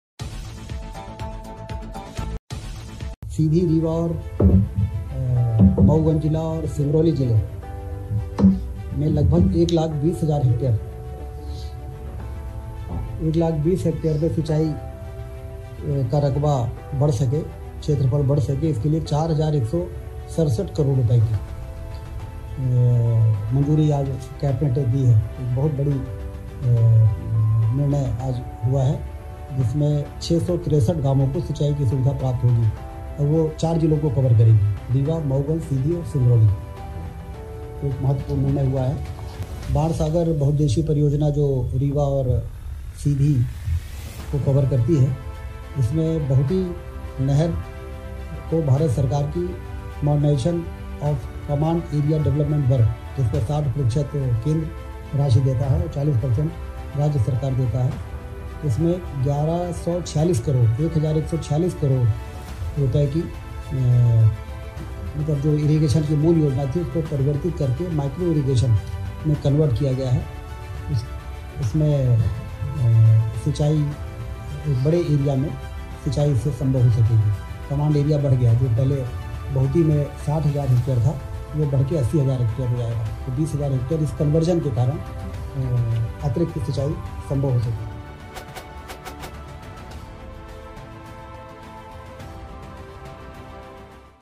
MP बैठक में विंध्य क्षेत्र को मिली सौगात के लिए आदरणीय PM श्री जी और माननीय मुख्यमंत्री जी का आभार
मध्यप्रदेश कैबिनेट बैठक में विंध्य क्षेत्र को मिली सौगात के लिए आदरणीय प्रधानमंत्री श्री @narendramodi जी और माननीय मुख्यमंत्री @DrMohanYadav51 जी का आभार!

मध्य प्रदेश की कैबिनेट बैठक में विंध्य प्रदेश को मिली ऐसी सौगात की रातो रात विंध्य प्रदेश हो गया मालामाल